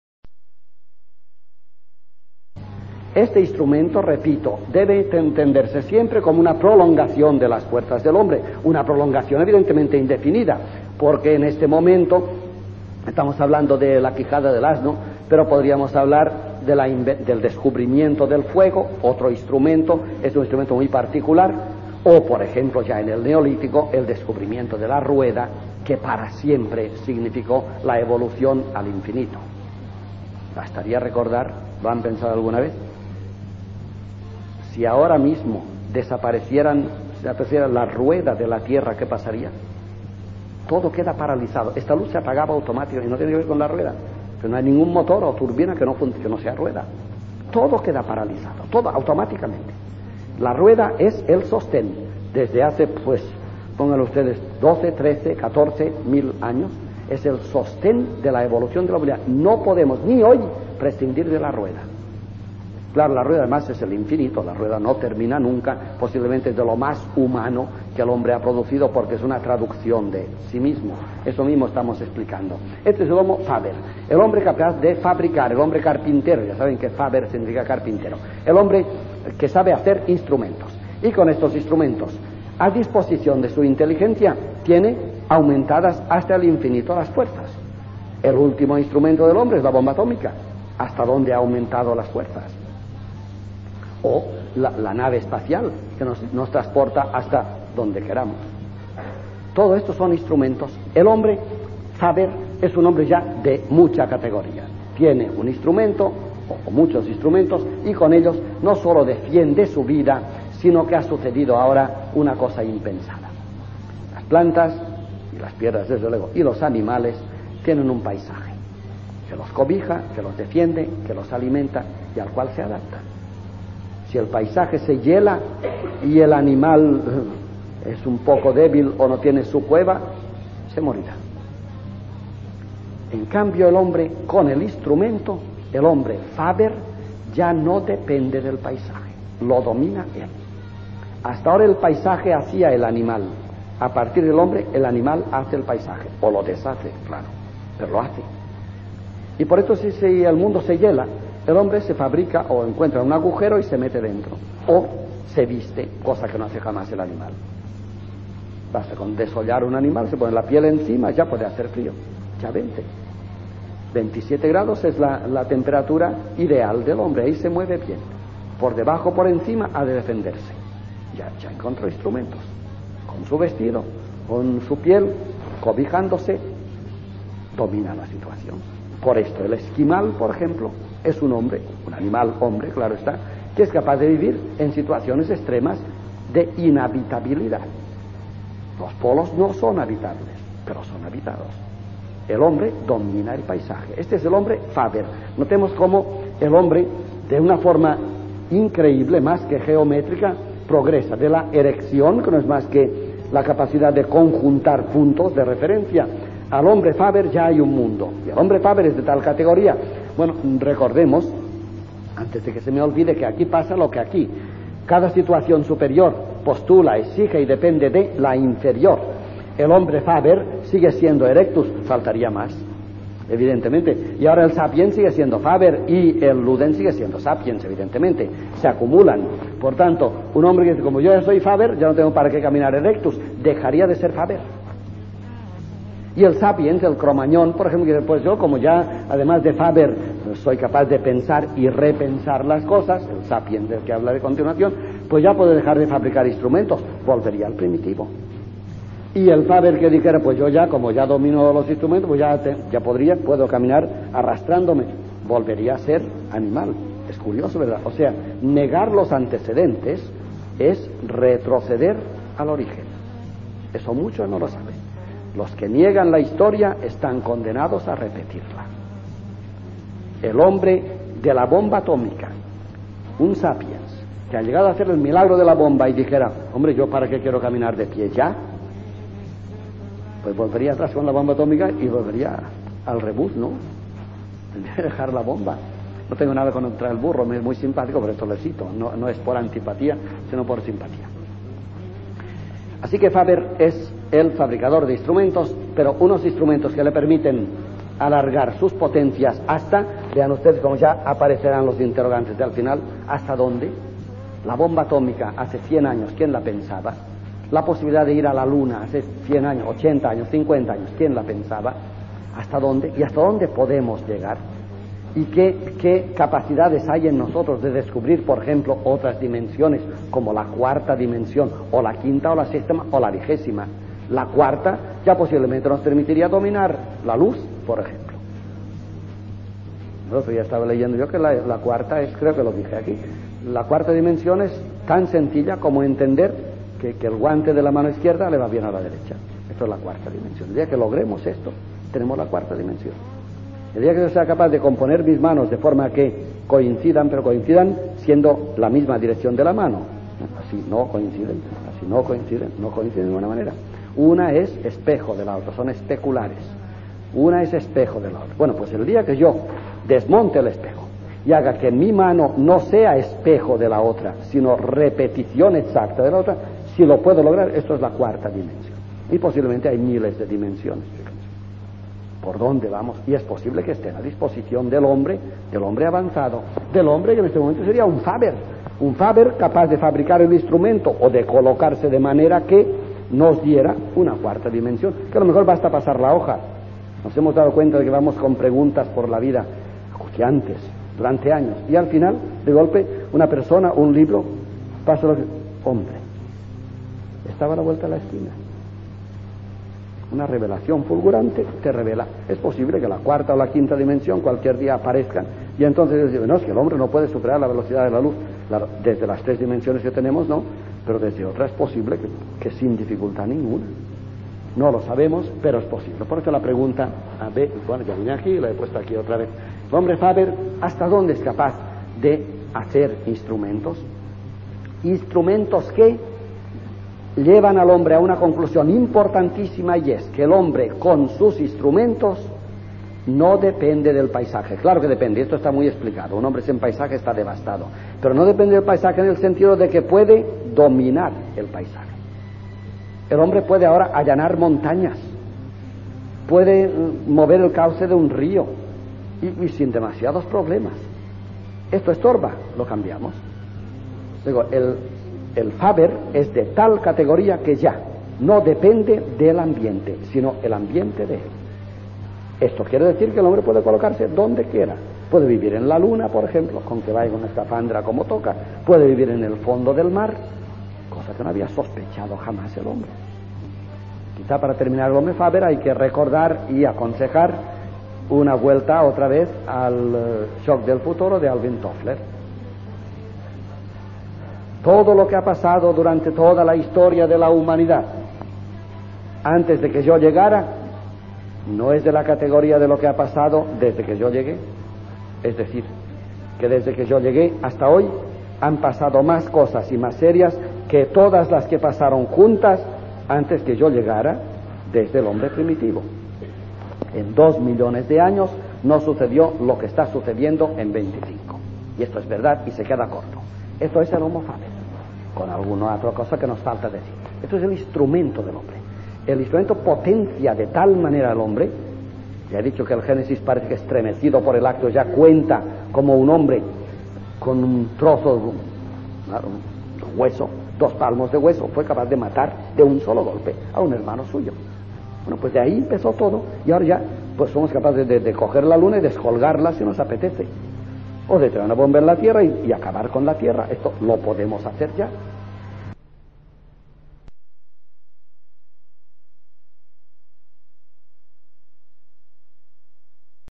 Curso del Padre Antonio Oliver L.11/11 Cap.3/7 Antropologia (El punto en que nos encontramos)
Lección 11. El punto en que nos encontramos. [22/Abr/88] (11 de 11)
03 homo faber

11.1. Una crisis como nunca se ha vivido (Spengler).
11.2. Las perspectiva de ser hombre de manera inédita.
11.3. La "postmodernidad" o el desengaño.
11.4. El Futuro no viene, se hace; la huída como solución.
11.5. El miedo a la libertad (Fromm) como posible suicidio.
11.6. Un desafío (Challenge-Response:Toynbee) para la esperanza.

01 Las cuatro etapas
02 homo erectus
03 homo faber
04 homo sapiens
05 sapiens sapiens
06 los productos de sapiens
07 el homo ludens y el amor